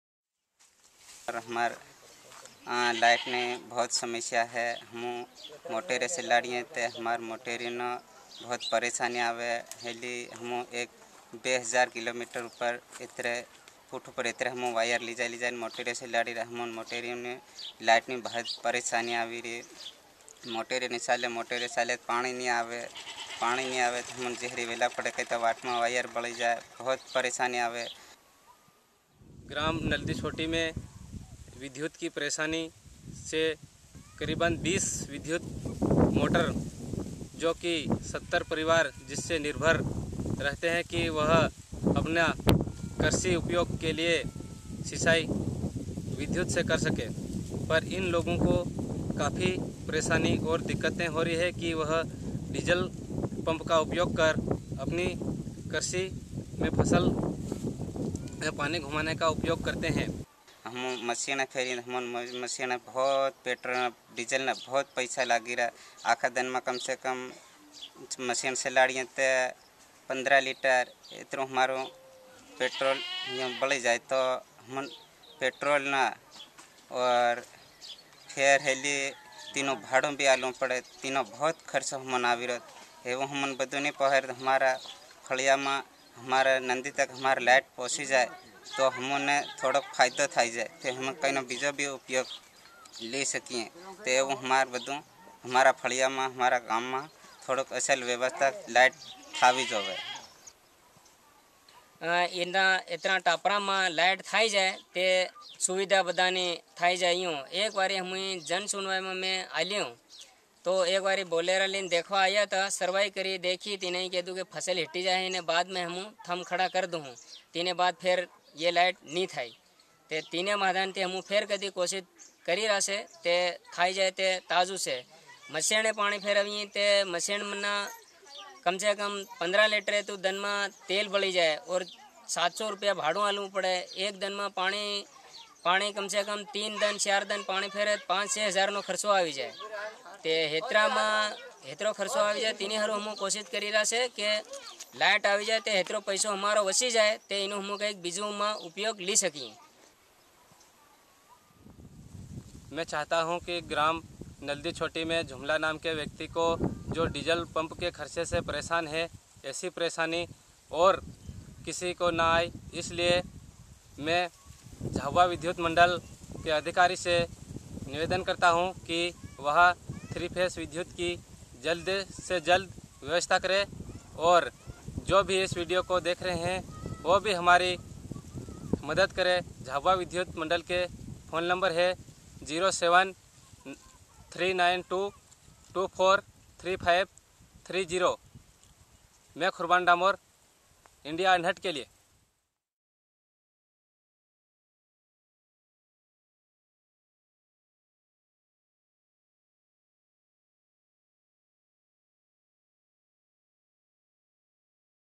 Non existing of the electricity, farmers crops are getting damaged - Kurban reports for IndiaUnheard
Community correspondent Kurban Dampr reports from Madhya Pradesh for Video Volunteers.